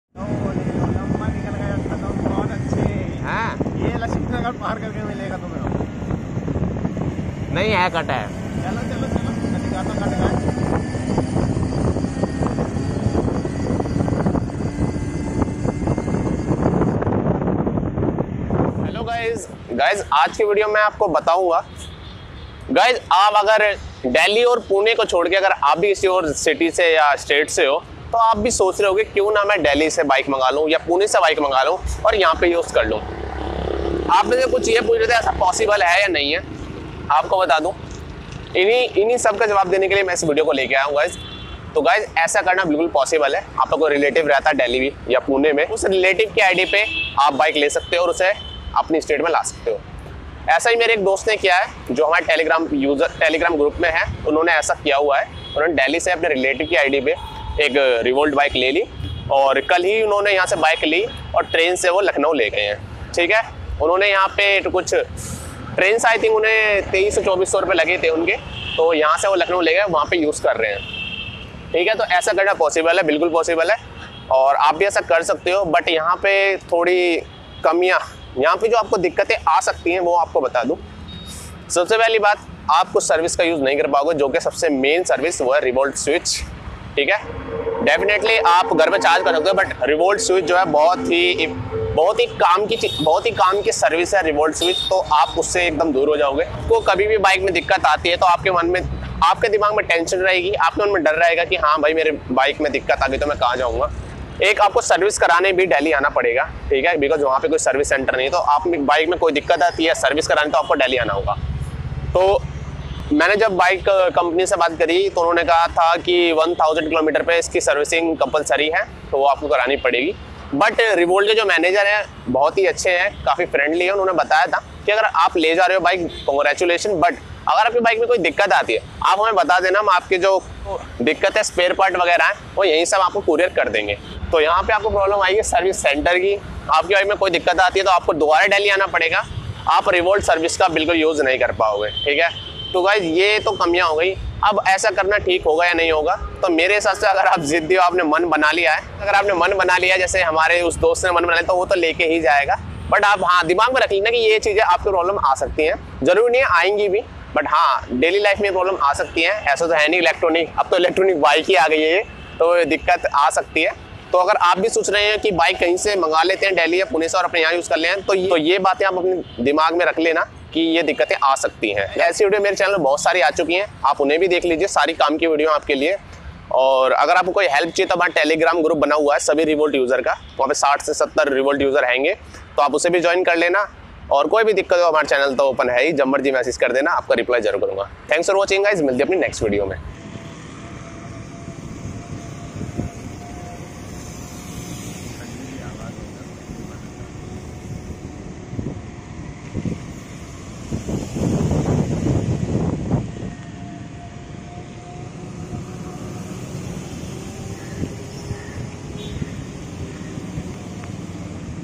Is it good idea to bring revolt from other state? | (दूसरे राज्य से revolt लाना ठीक होगा?)
hello doston Aaj is video mein maine aapko bataya hai ki kaise aap revolt delhi se lekar apni city me chala sakte h ,  video Pasand Aaye To like kar dena thanks for watching

2)All 4 sound of revolt rv400 test on road

3)Belt sound in revolt rv400

Revolt bike telegram group